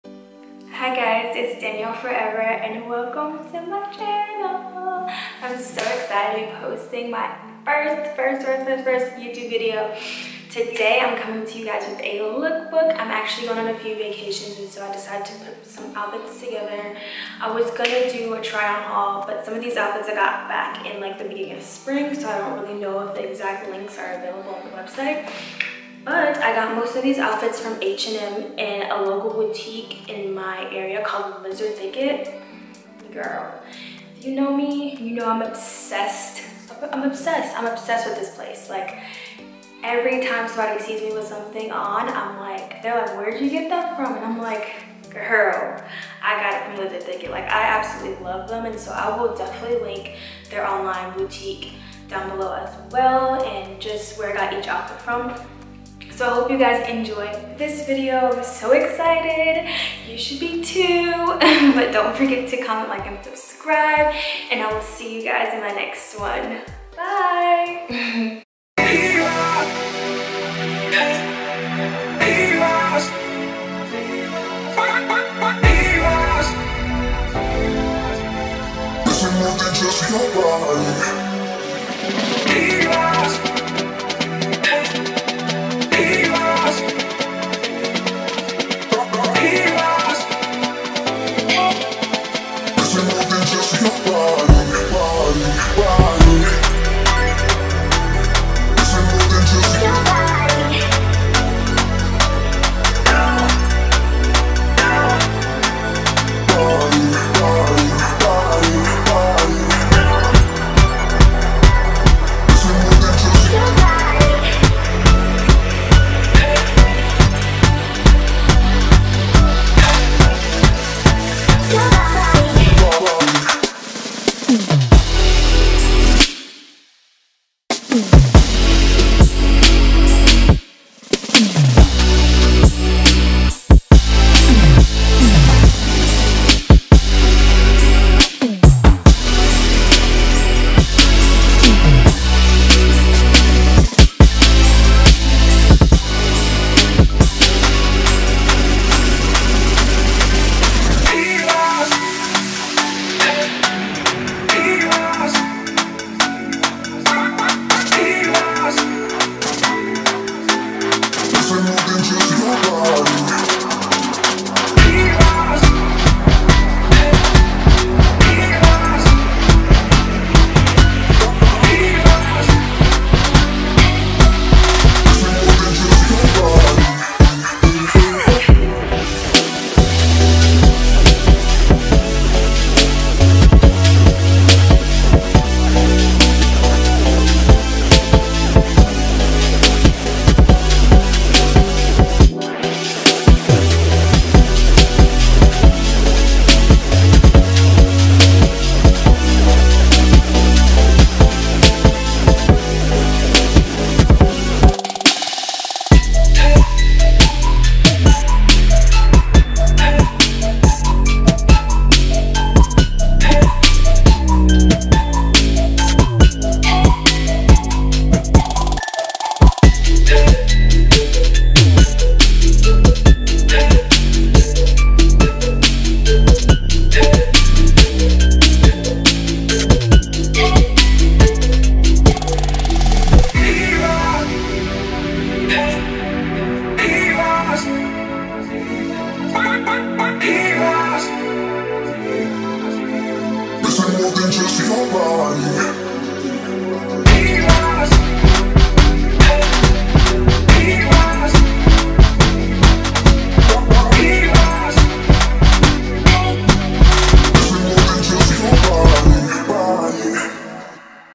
SUMMER LOOKBOOK 〡VACATION OUTFITS
If you have any video request...you're welcomed to can leave a comment of what you're interested in seeing.

*OUTFIT 3: Floral short overalls with off the shoulder top

*OUTFIT 5: Black and white stripe maxi romper
TJ Max

*OUTFIT 7: Cropped long sleeve with black denim skirts and chain belt